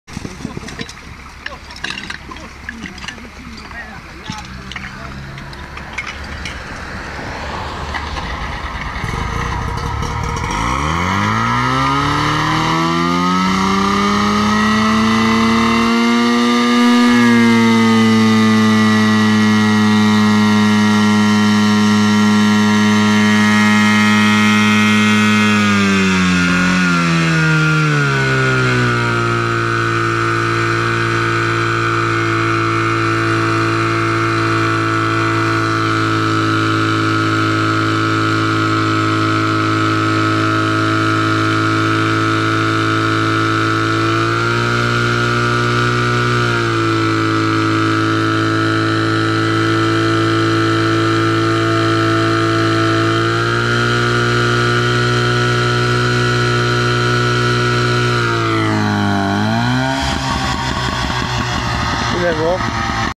Trening OSP JANOWKA przed zawodami [29.06.2011]
Pierwsza próba OSP JANÓWKA w pełnym umundurowaniu przed zawodami które mają odbyć się dnia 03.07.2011 w Rozprzy